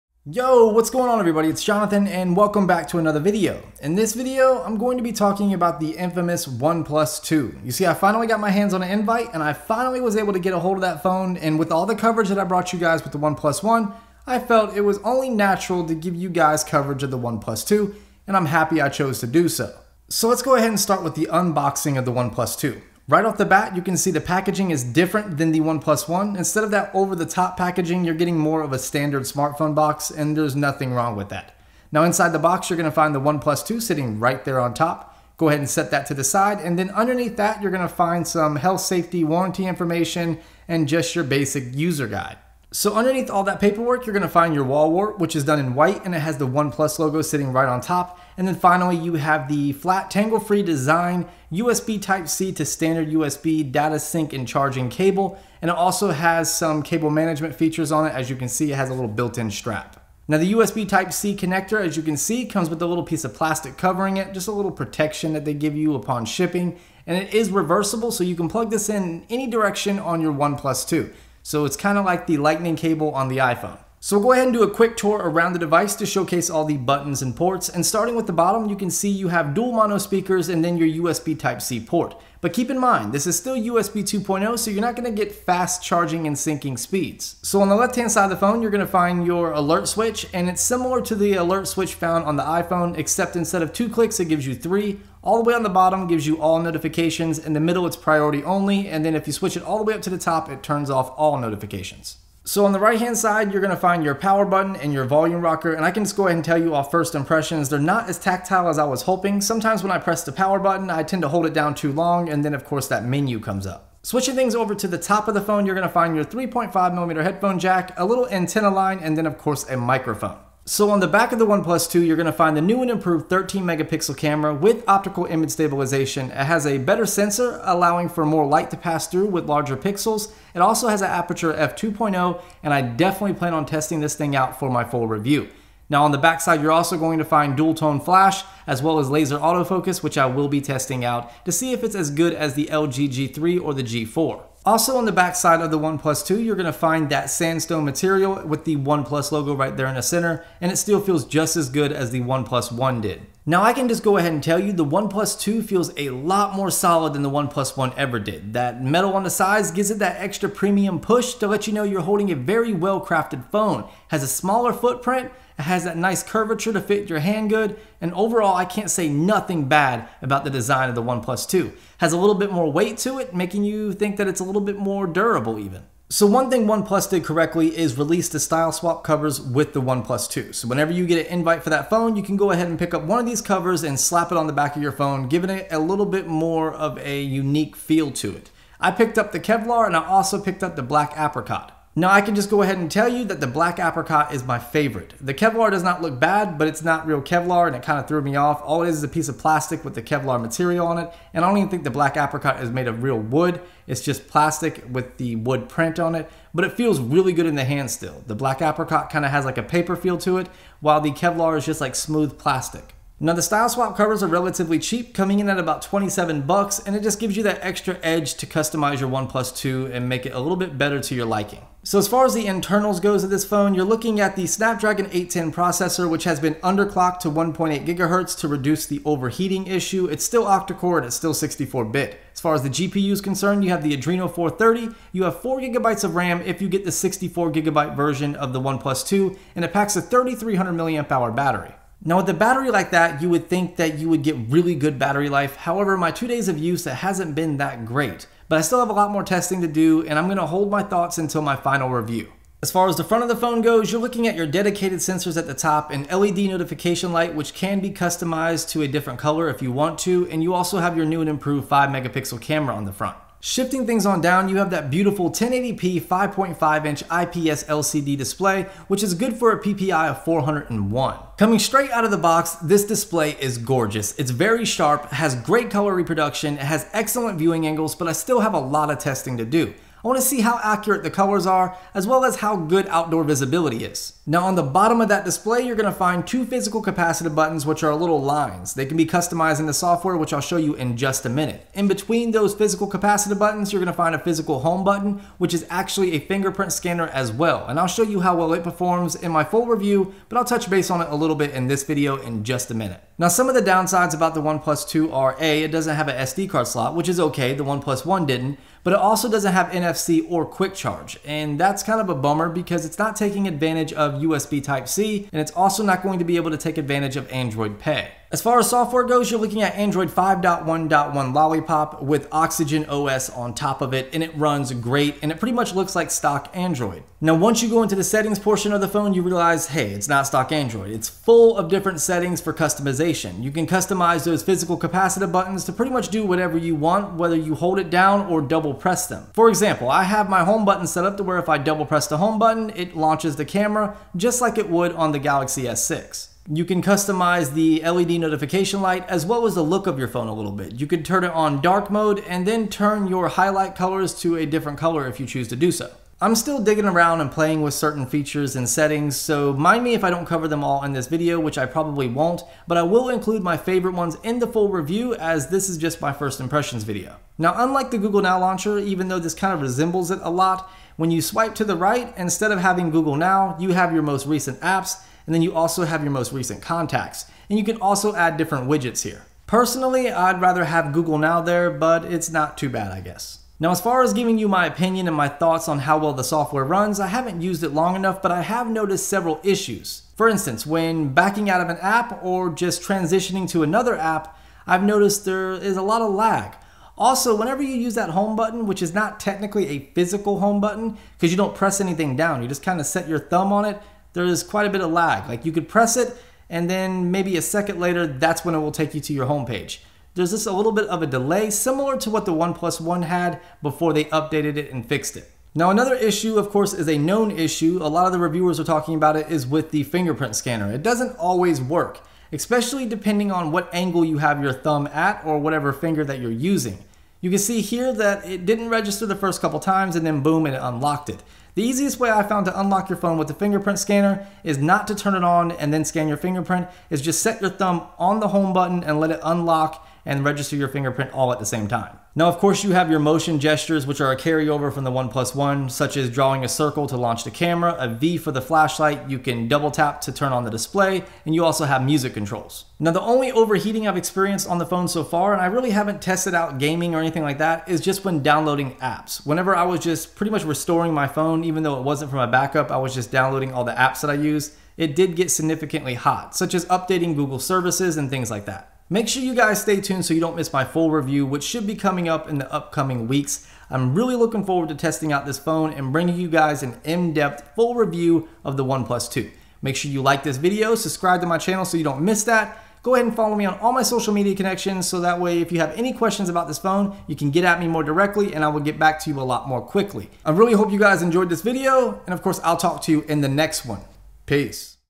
OnePlus 2 First Impressions and Overview + Style Swap Covers
The OnePlus 2 is a device that most want but can't get. Luckily, OnePlus hit me up with an invite so I could bring you guys coverage. In this video I will give you guys my first impressions of the 2016 "Flagship Killer".

My Gear and Other Items